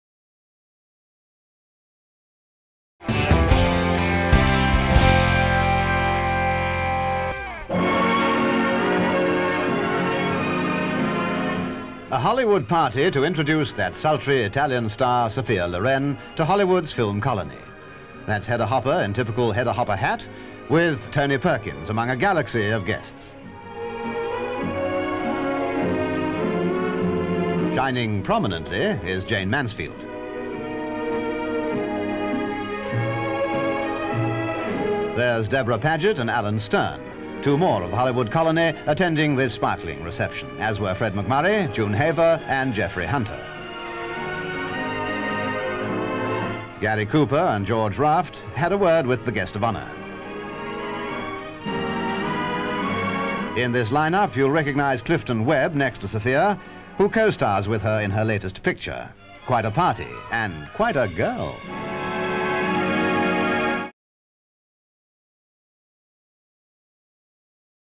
On This Day: 29 April 1957
In 1957, Sophia Loren was introduced to Hollywood stars at a party welcoming her to Tinsel Town. (April 29)